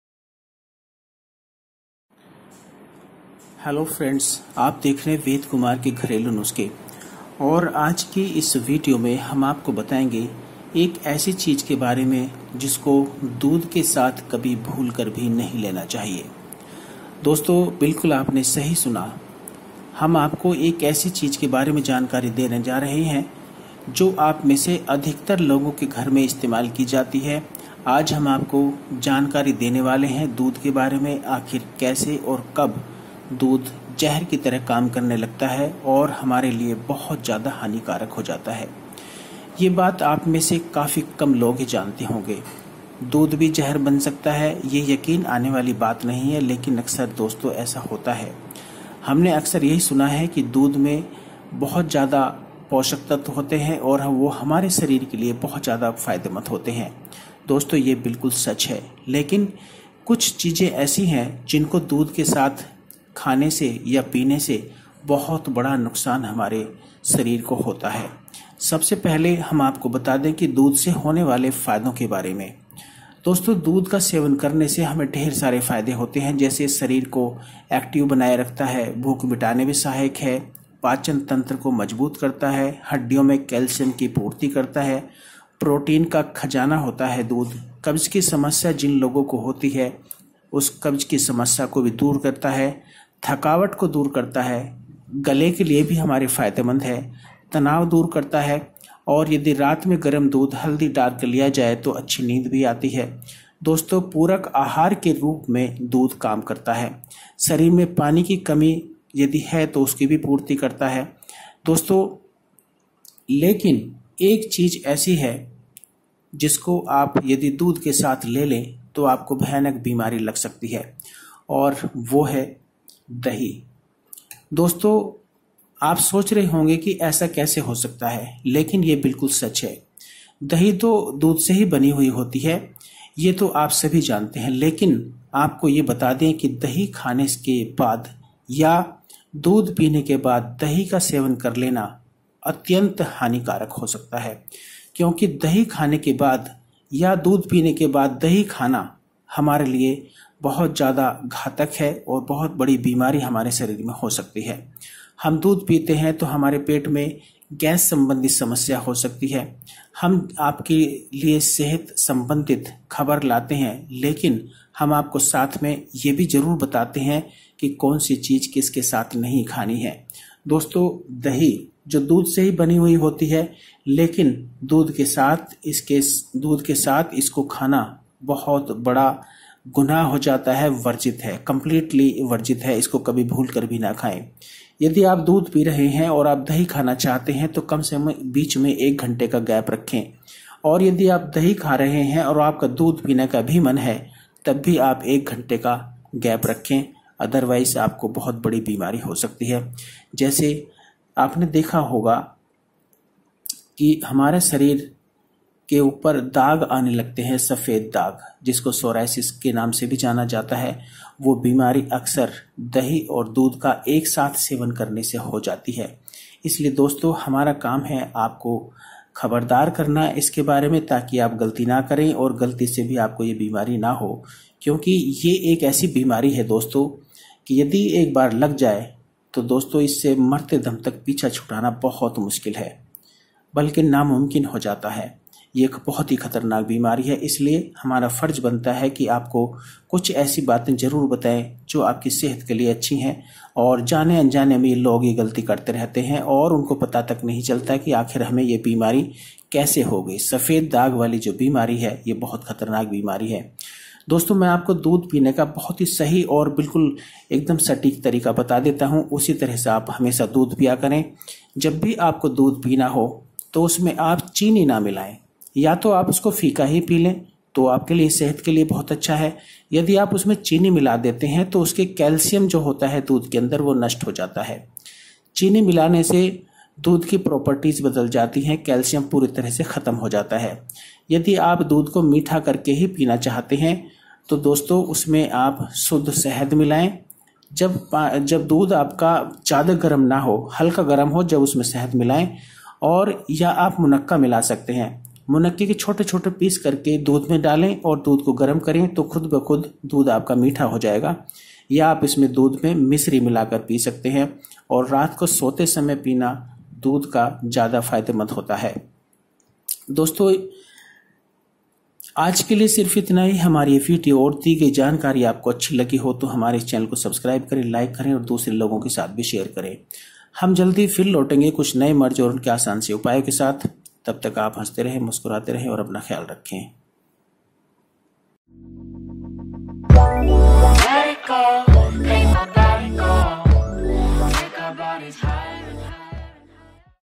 दूध पीने के बाद भूलकर भी ना करें इस चीज का सेवन, वरना हो सकती है यह बीमारी /Never Eat this With Milk
दूध पीने के बाद भूलकर भी ना करें इस चीज का सेवन, वरना हो सकती है यह बीमारी / Never Eat this Foods With Milk / Most Harmful Food Combinations